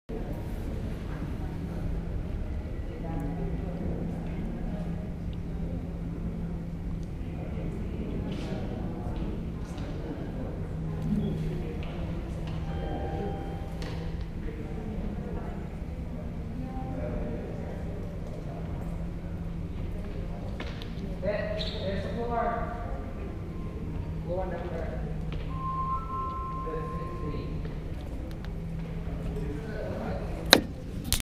GoPro Hero4 4K Raw Video
Video taken from Chase Tower in Downtown Houston.